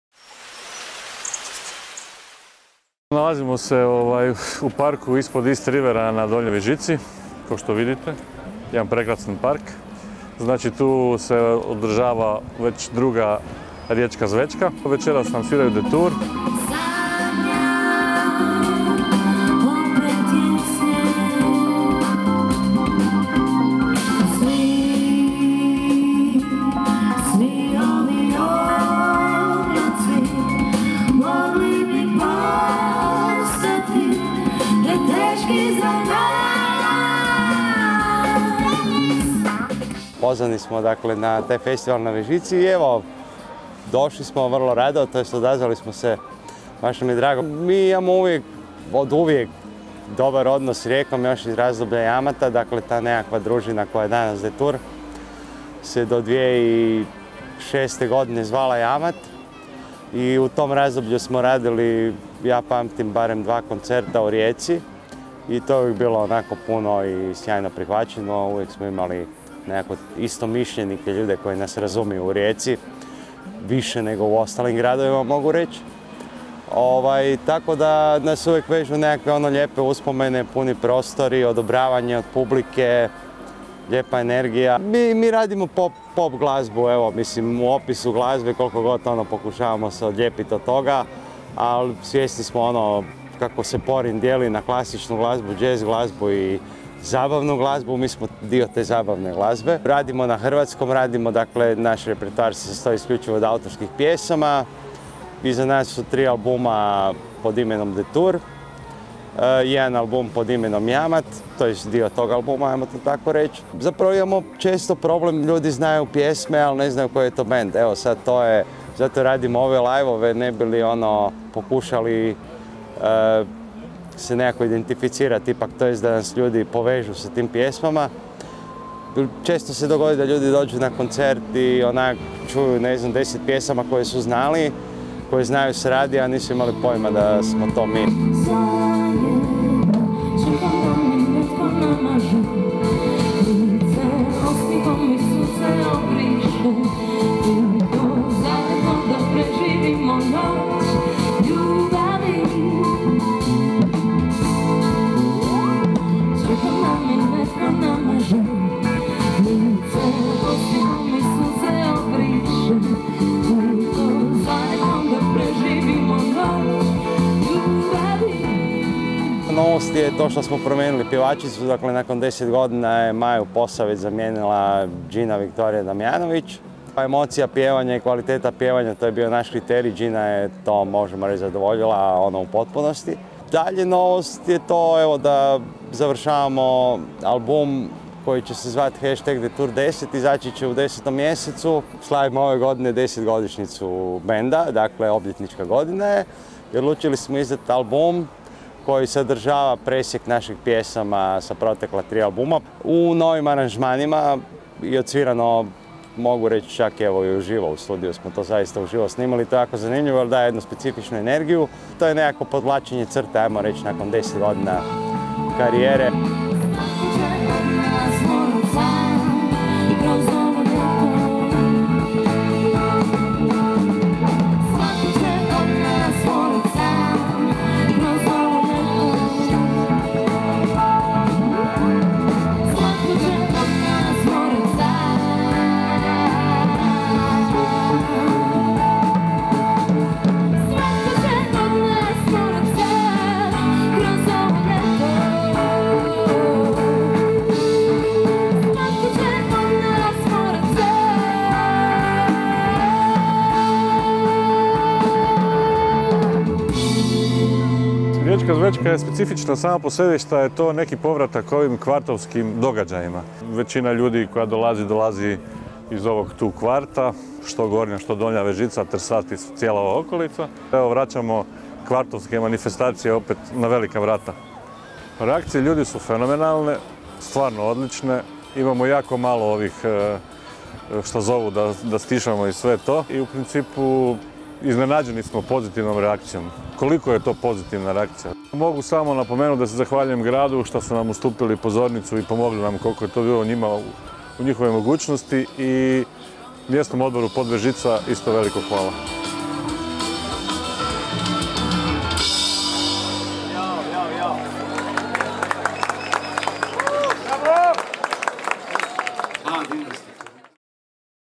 MojaRijeka.hr - Riječka zvečka: Detour rasplesao riječku publiku
U sklopu drugog izdanja Riječke zvečke, u parku na Vežici, 28. srpnja, 2016., nastupio je bend Detour.